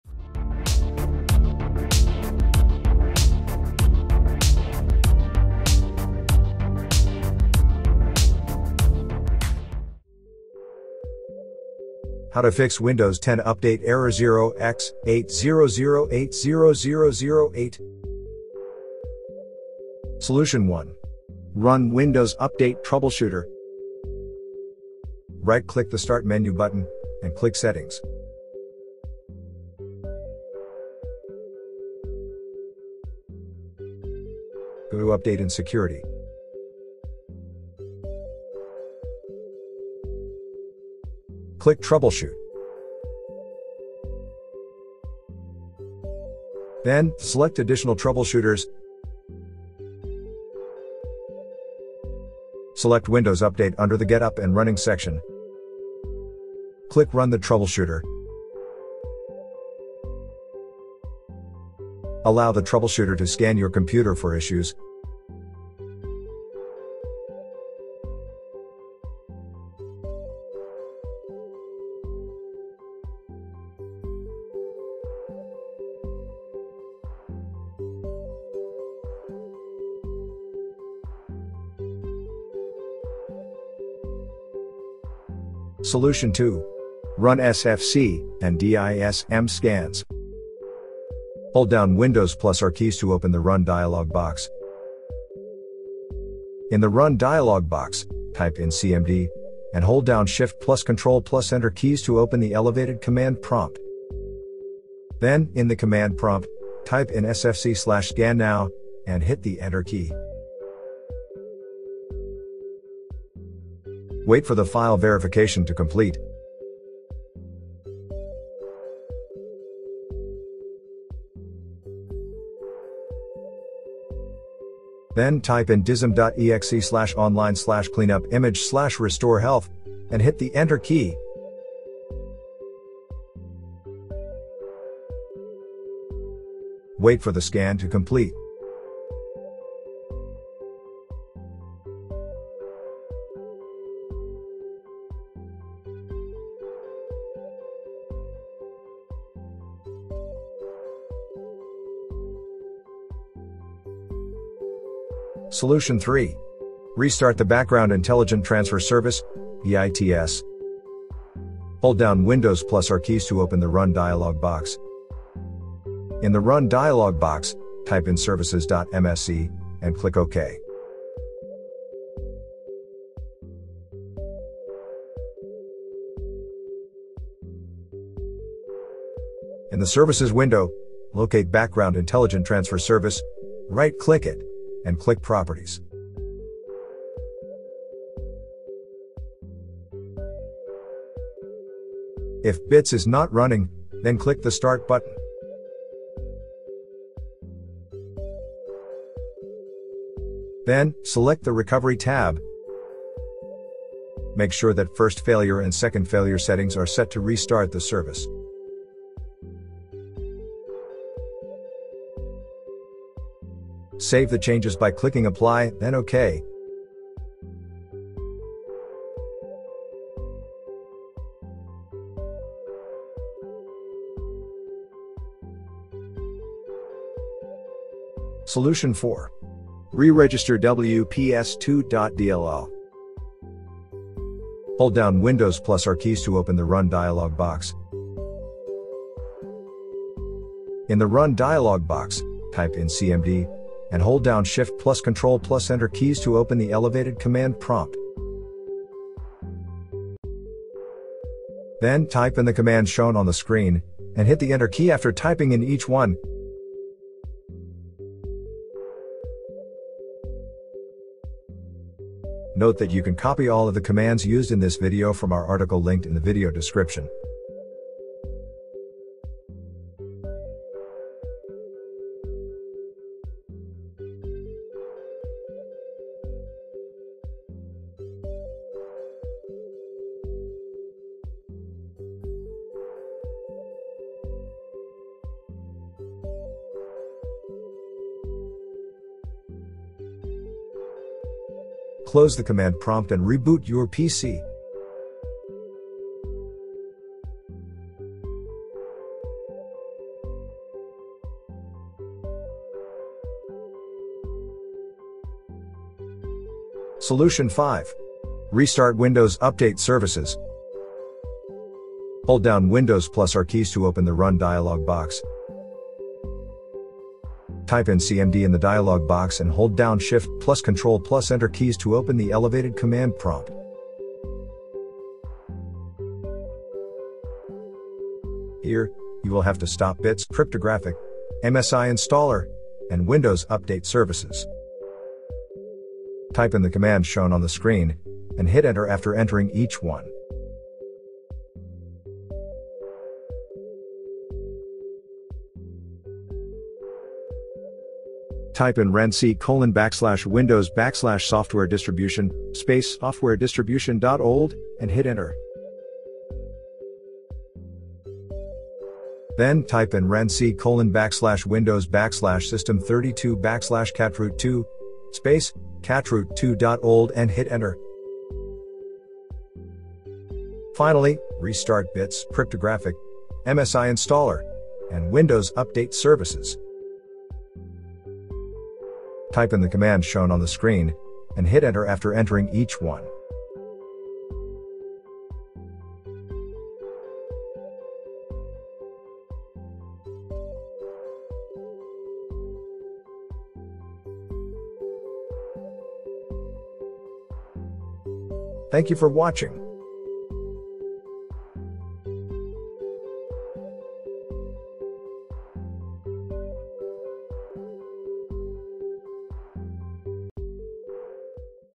How to Fix Windows 10 Update Error 0x80080008?
Video showing how to Fix Windows 10 Update Error 0x80080008.